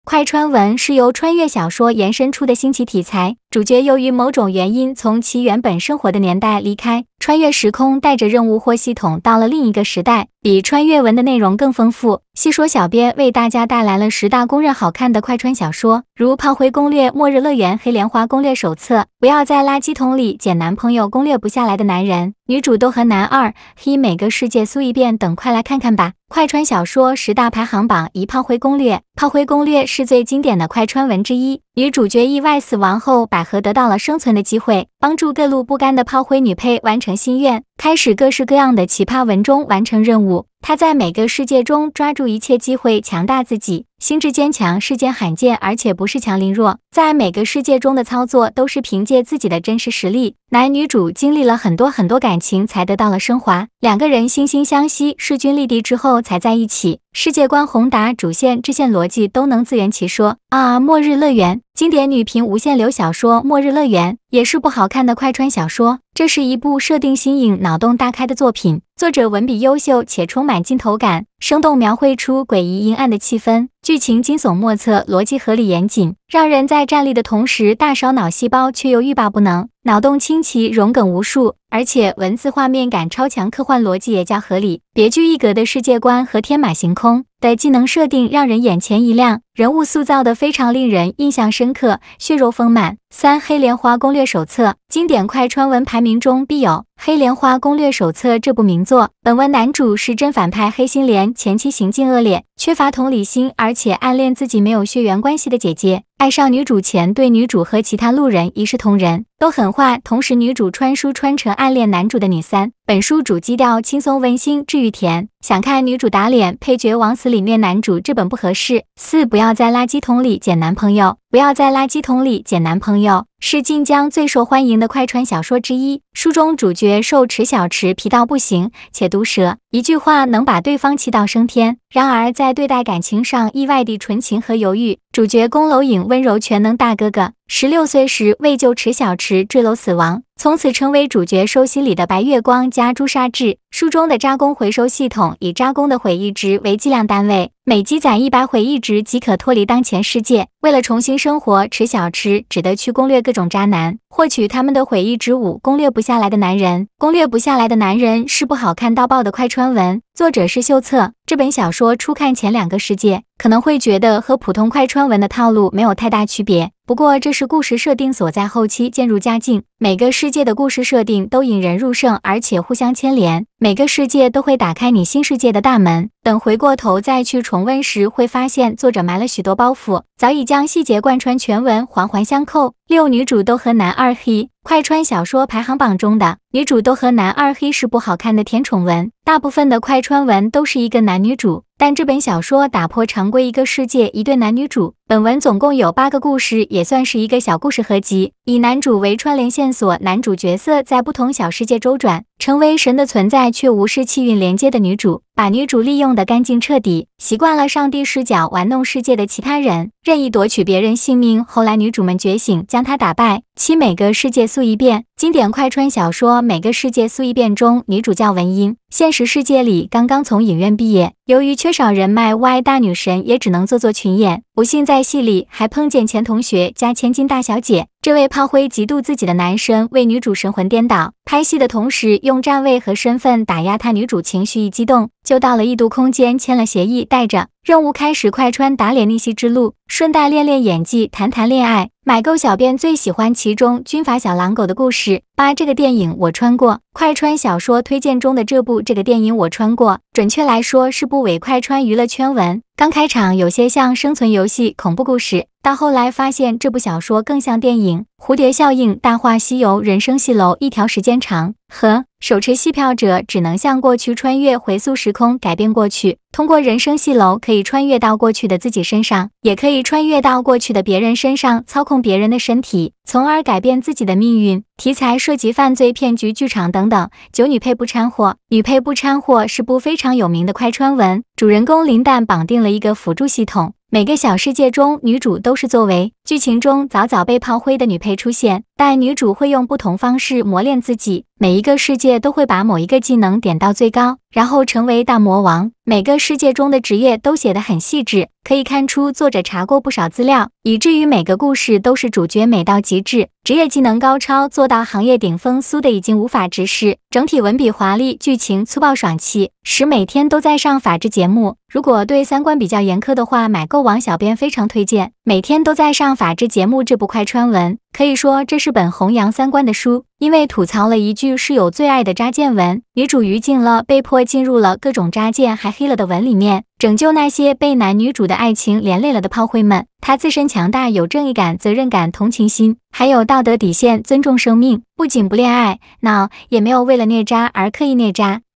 快穿小说十大排行榜 十大公认好看的快穿小说排行榜
快穿小说十大排行榜

1、炮灰攻略
2、末日乐园
3、黑莲花攻略手册
4、不要在垃圾桶里捡男朋友
5、攻略不下来的男人
6、女主都和男二HE
7、每个世界苏一遍
8、这个电影我穿过
9、女配不掺和
10、每天都在上法制节目

各大文体娱乐排行榜，欢迎讨论指正。

感谢平台，感谢关注，记得点个赞哟！